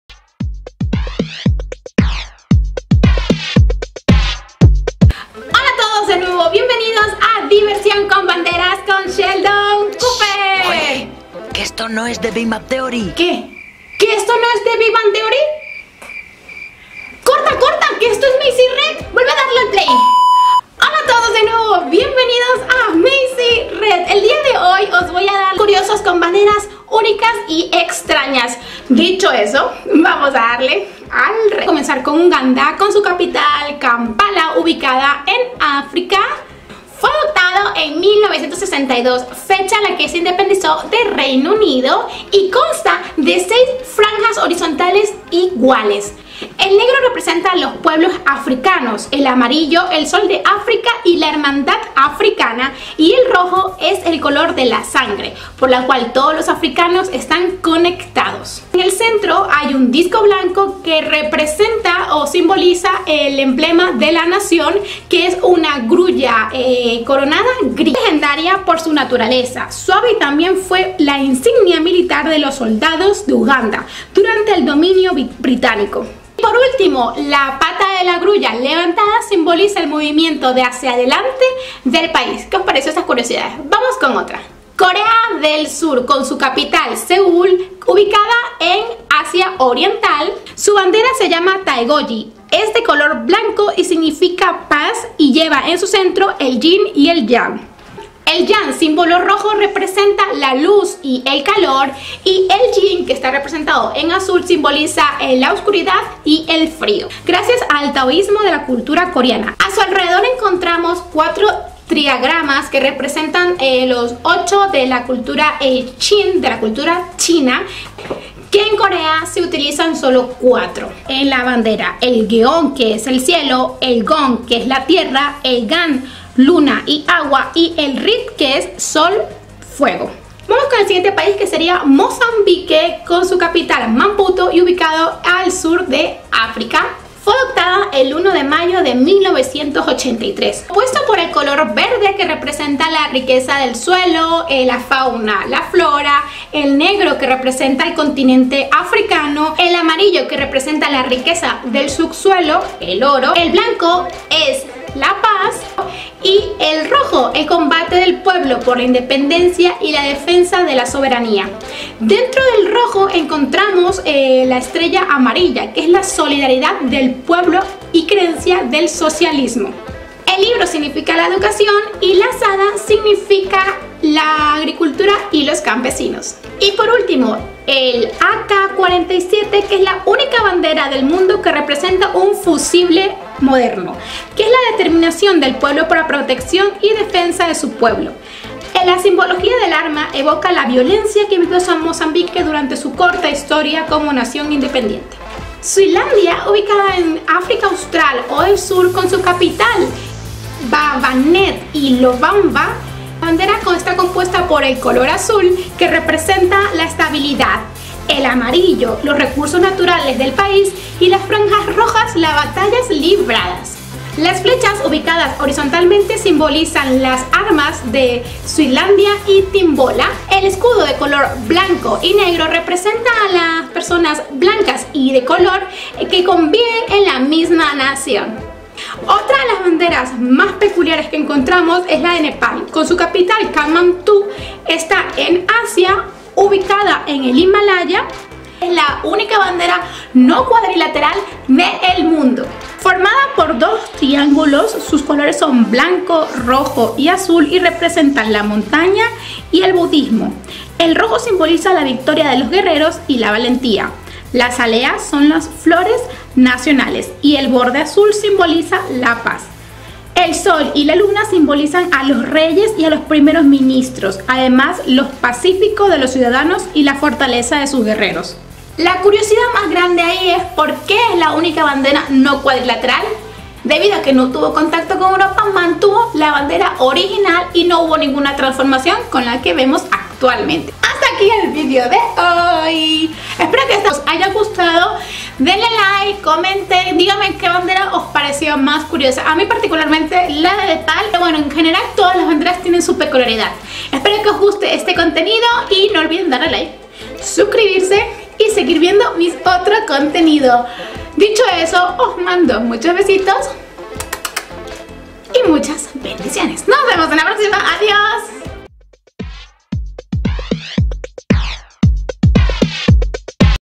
Curiosidades de banderas | Fun with flags
Buenas!!!
Hoy os dare las 5 banderas de las 194 que existen de cada país, voy hacer un mini serie para que podamos saber mas sobres las banderas que vemos.

En este caso:
Nepal
Mozambique
Corea del Sur
Suazilandia
Uganda